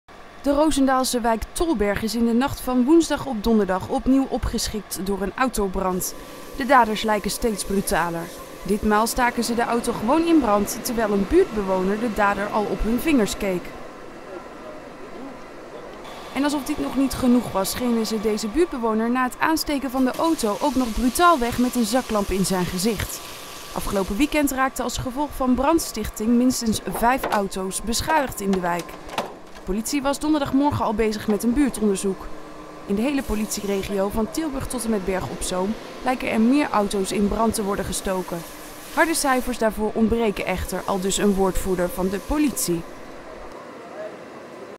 Brandstichters hondsbrutaal
De dadaders van brandstichting in een  auto in Roosendaal zijn hondsbrutaal. Ze staken de over de auto gegoten brandbare vloeistof aan terwijl een getuige stond toe te kijken.